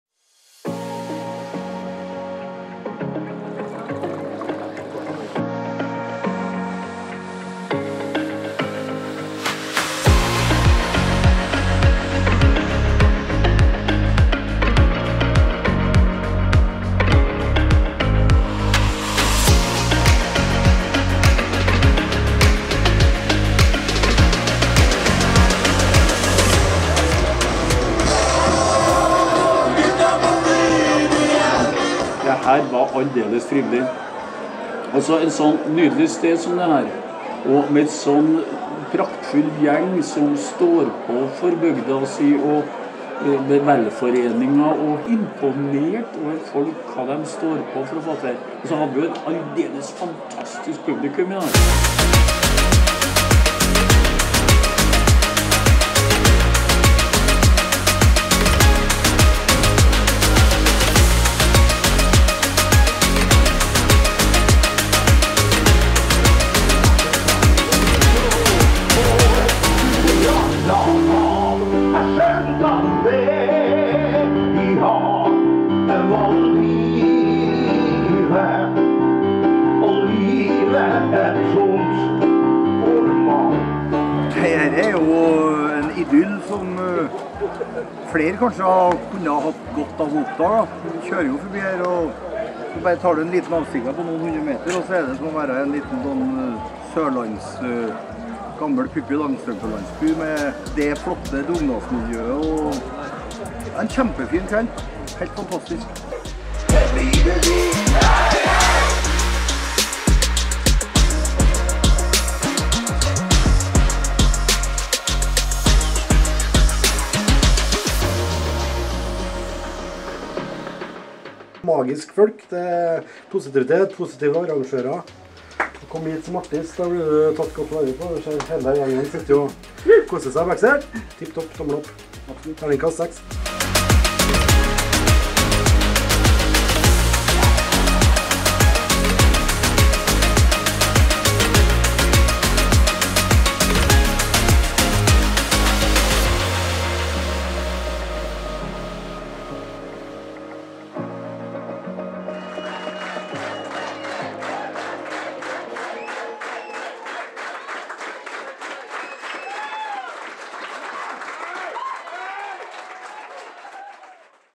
ATMOSFÆRE & STEMNING · HYLLA VEL (WOW Film As)
Hylla Vel er en velforening for beboere på Hylla på Røra. Hovedmål er å ordne trivsel og samhold mellom beboerne og å presentere boplassen vår som det fine stedet det er. Sjekk ut info under og ta gjerne kontakt. For mer om Hylla Vel se nettside